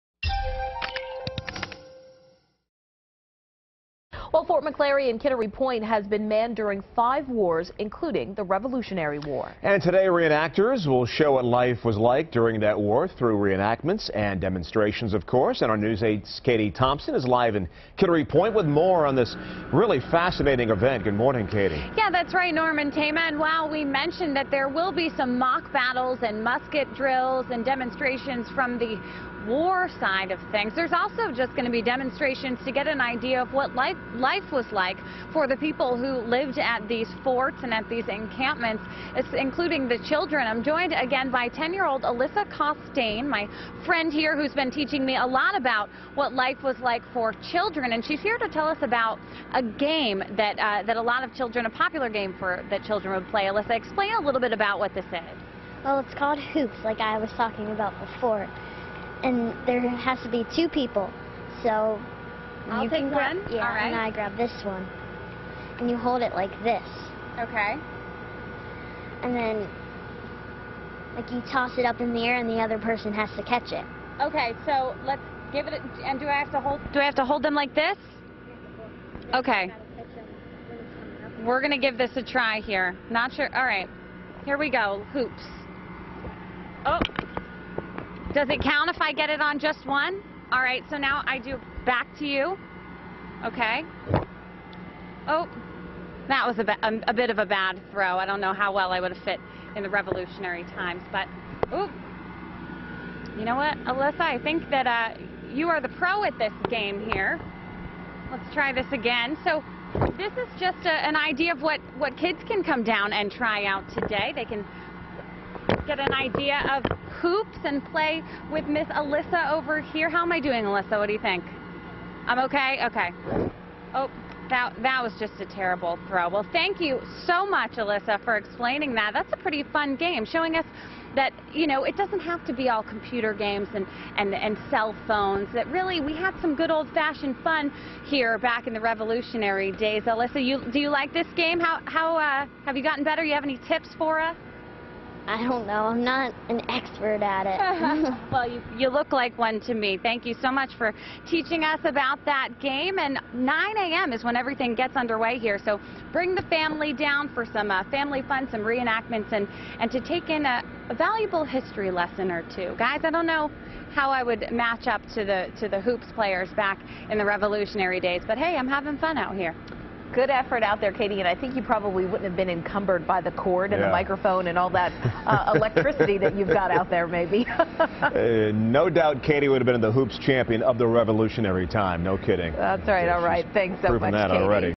Re-enactors holding Revolutionary War encampment at Fort McClary
Re-enactors and demonstrators with the Friends of Fort McClary are sponsoring a Revolutionary War Encampment where they will be offering spectators a glimpse at life during that time period. WMTW News 8's Katie Thompson has a preview.